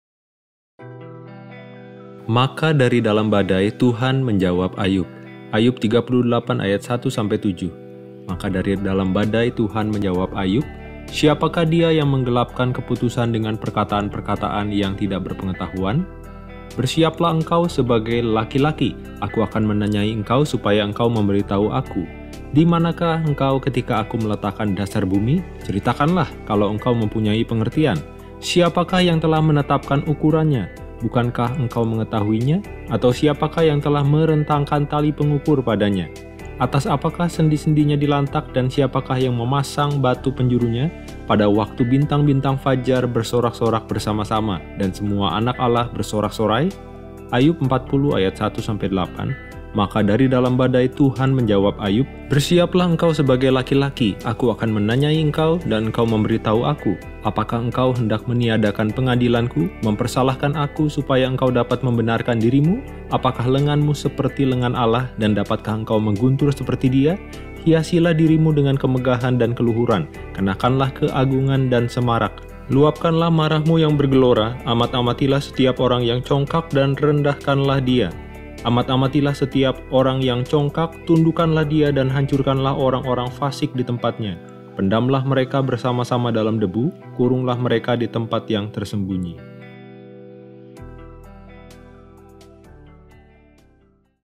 034 Februari 03 - Maka Dari Dalam Badai TUHAN Menjawab Ayub - Alkitab Satu Menit Sepanjang Tahun
Ayub 38:1-7
38:1 Maka dari dalam badai TUHAN menjawab Ayub:
38:2 "Siapakah dia yang menggelapkan keputusan dengan perkataan-perkataan yang tidak berpengetahuan?
38:3 Bersiaplah engkau sebagai laki-laki! Aku akan menanyai engkau, supaya engkau memberitahu Aku.
38:4 Di manakah engkau, ketika Aku meletakkan dasar bumi? Ceritakanlah, kalau engkau mempunyai pengertian!
38:5 Siapakah yang telah menetapkan ukurannya? Bukankah engkau mengetahuinya? -- Atau siapakah yang telah merentangkan tali pengukur padanya?
38:6 Atas apakah sendi-sendinya dilantak, dan siapakah yang memasang batu penjurunya
38:7 pada waktu bintang-bintang fajar bersorak-sorak bersama-sama, dan semua anak Allah bersorak-sorai?

Ayub 40:1-8
40:1 Maka dari dalam badai TUHAN menjawab Ayub:
40:2 "Bersiaplah engkau sebagai laki-laki; Aku akan menanyai engkau, dan engkau memberitahu Aku.
40:3 Apakah engkau hendak meniadakan pengadilan-Ku, mempersalahkan Aku supaya engkau dapat membenarkan dirimu?
40:4 Apakah lenganmu seperti lengan Allah, dan dapatkah engkau mengguntur seperti Dia?
40:5 Hiasilah dirimu dengan kemegahan dan keluhuran, kenakanlah keagungan dan semarak!
40:6 Luapkanlah marahmu yang bergelora; amat-amatilah setiap orang yang congkak dan rendahkanlah dia!
40:7 Amat-amatilah setiap orang yang congkak, tundukkanlah dia, dan hancurkanlah orang-orang fasik di tempatnya!
40:8 Pendamlah mereka bersama-sama dalam debu, kurunglah mereka di tempat yang tersembunyi.